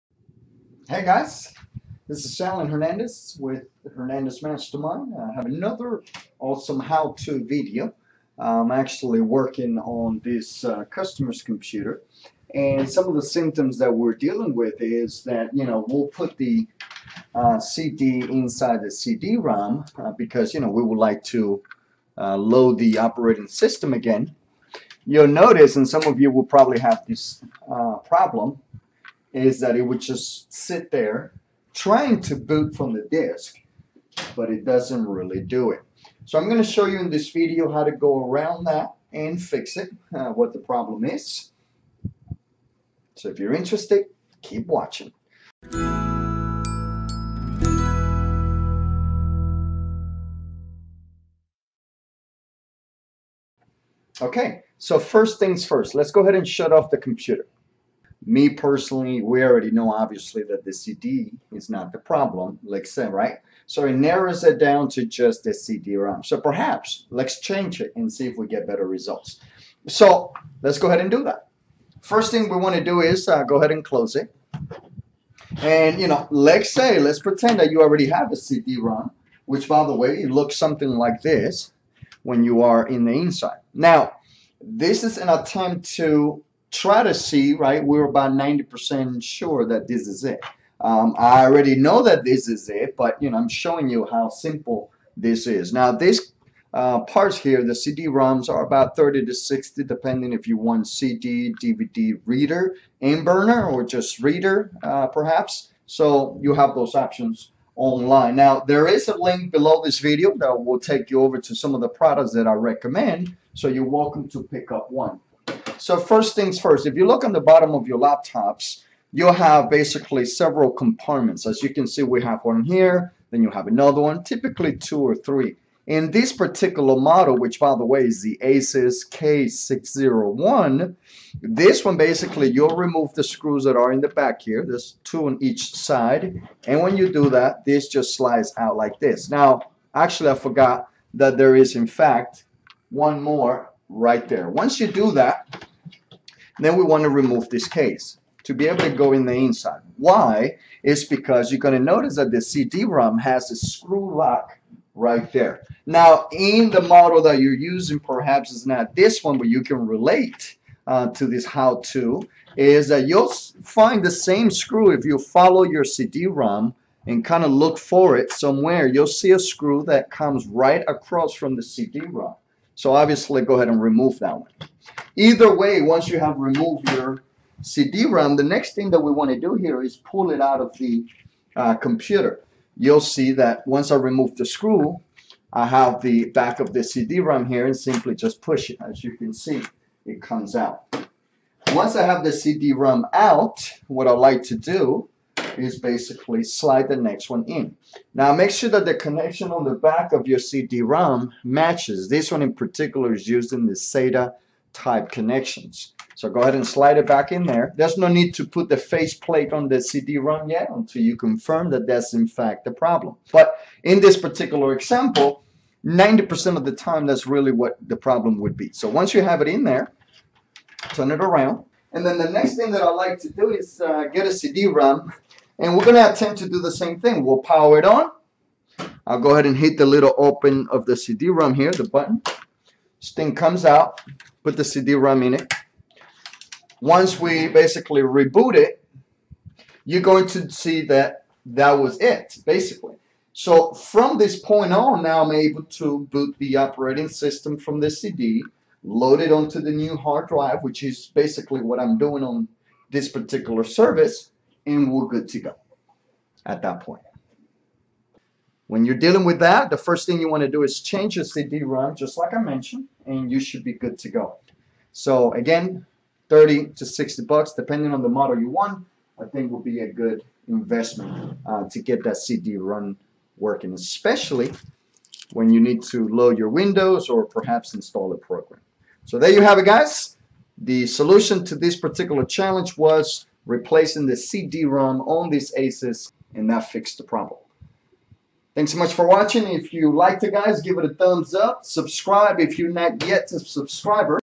How to Replace the CDROM on your Asus K60I Laptop
When you discover that your laptop cdrom is not working, it may be then time to replace it with a new one; a lot of times the lens decide to no longer read your cds.

Learning how to remove a cdrom from a laptop is one of the easiest parts to take out. Begin by finding the screw holding the cdrom in place; normally it would be the screw right across from it.

Products I Recommended from Amazon

Anyone with a screw driver should be able to install a cd/dvd rom on a laptop. Sometimes with a single screw and other times you may need to remove the covers in the way.

You will see that some cdrom products come with sata like connection, and other type of dvdrom offer the ide connection; make sure to purchase a replacement part that uses the same connection as your old laptop cdrom.

Most of the time trying to repair these cdrom type drives are not worth the cost or the time involved, these computer parts are typically no more than $60 bucks; installing a new cd/dvd drive on your laptop may just be the best move.

Regardless of the Asus laptop you are using, cd/dvd rom type drives are installed most of the time the same way; what might change is the connection  and the layout.